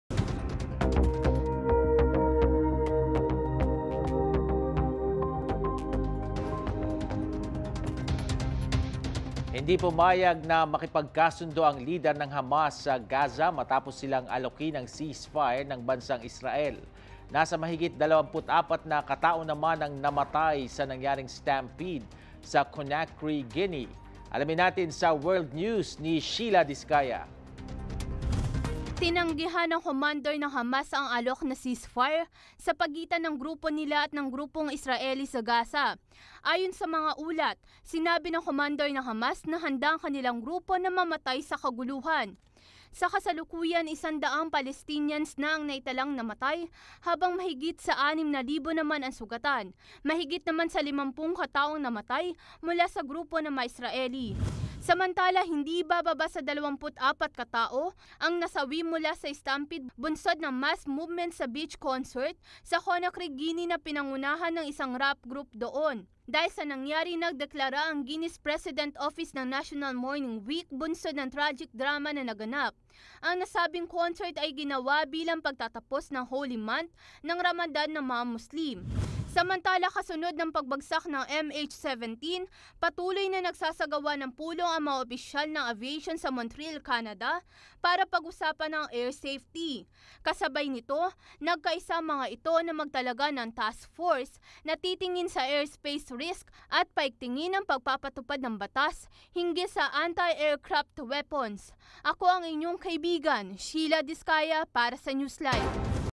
[World News] Hamas Commander refused ceasefire
[World News] Commander ng Hamas, tinanggihan ang ceasefire sa pagitan ng grupo nila at ng grupo ng Israeli

July 30, 2014 by Shiela Discaya